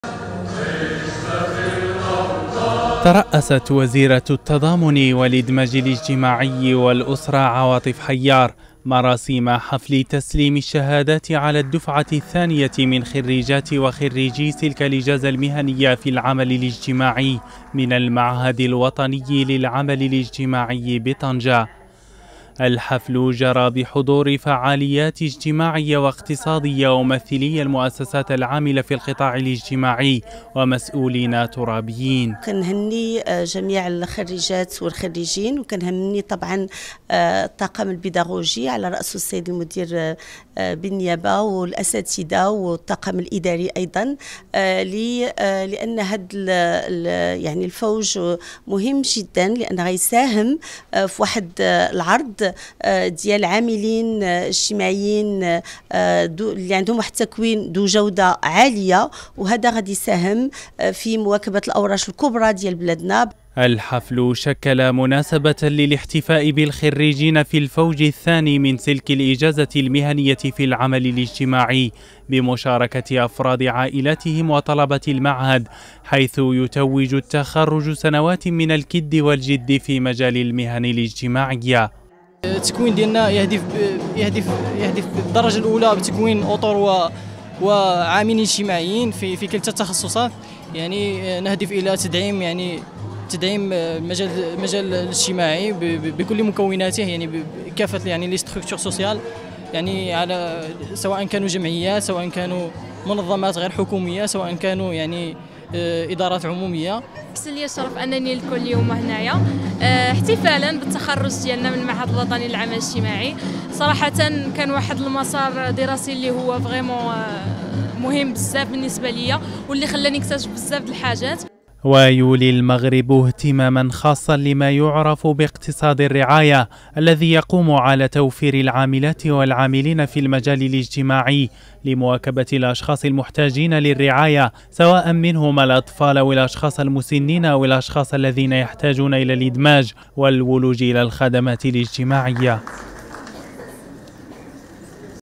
طنجة.. تخرج الفوج الثاني من سلك الإجازة المهنية في العمل الاجتماعي
لمتابعتنا

لتحميل التطبيق